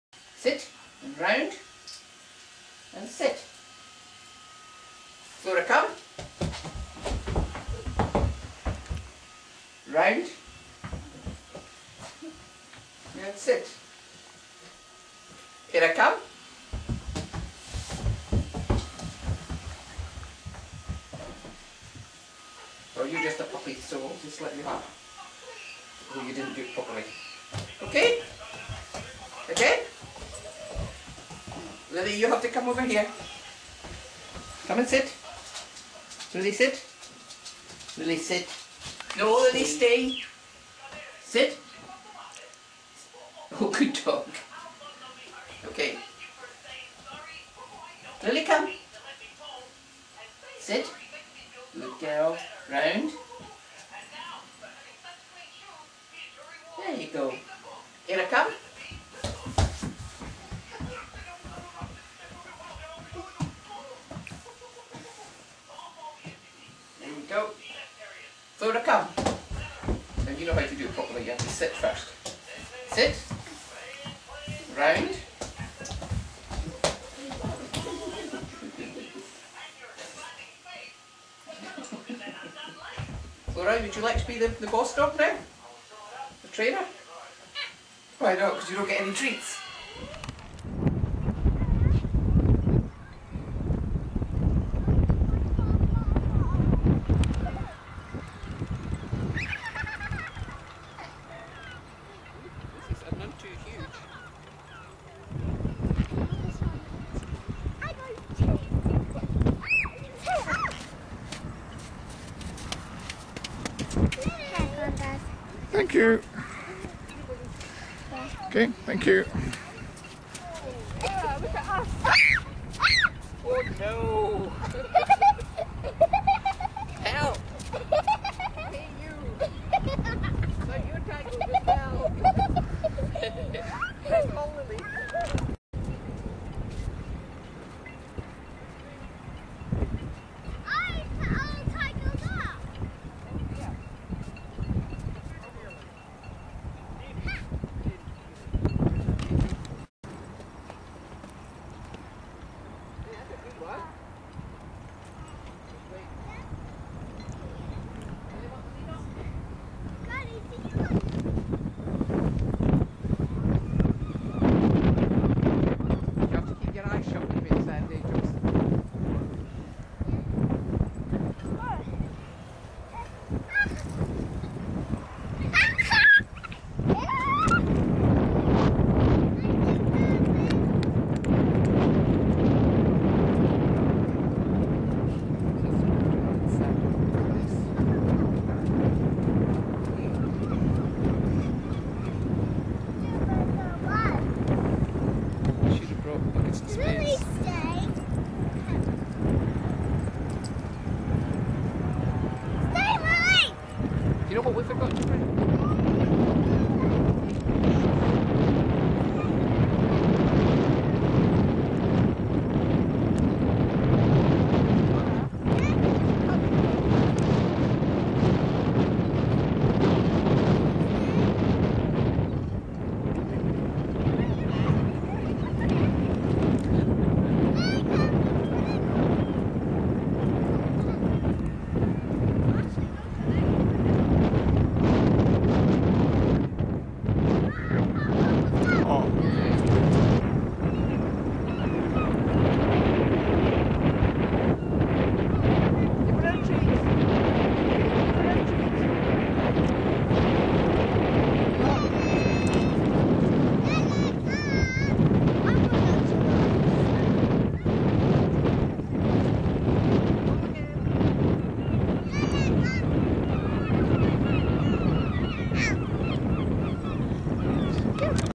The Girls at Hatton Easter 2010
Flora, Eira and Lilly have fun